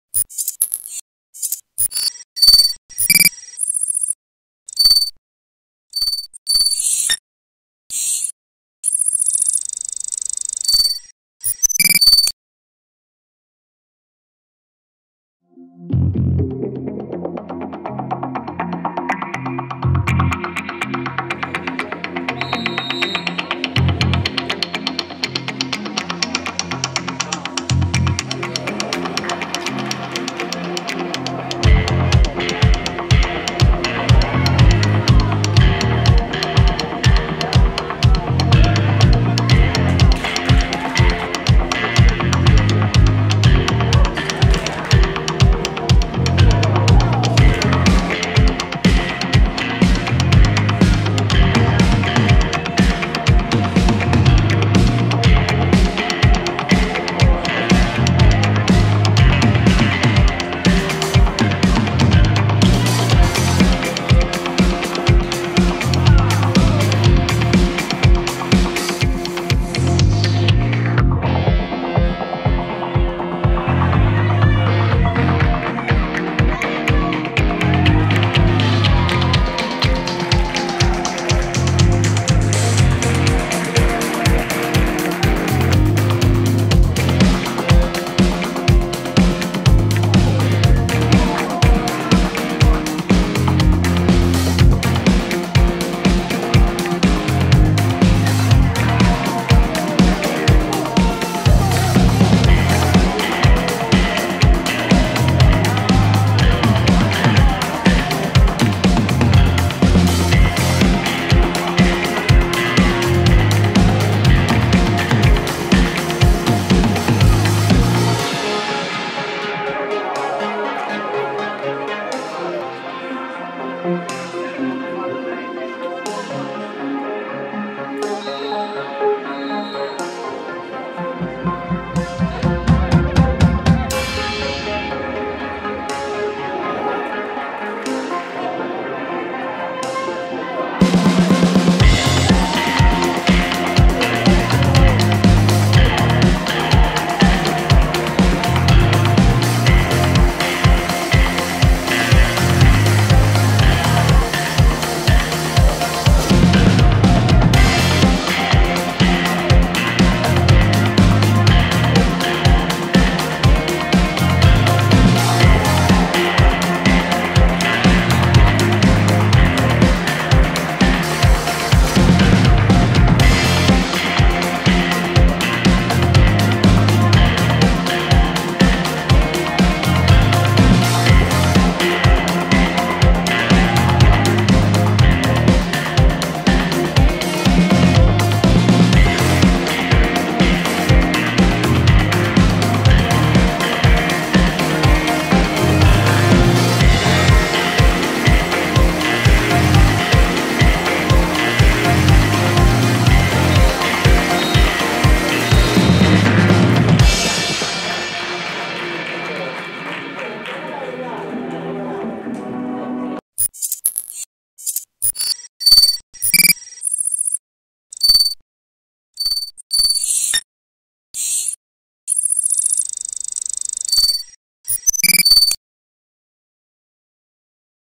SB08TV | SALERNUM BARONISSI-PRO SANGIORGESE 3-0 | GLI HIGHLIGHTS
Prima giornata della Coppa Italia Dilettanti fase regionale. Il Salernum Baronissi supera per 3-0 la Pro Sangiorgese e fa sua gara 1 del girone I.
La sintesi filmata.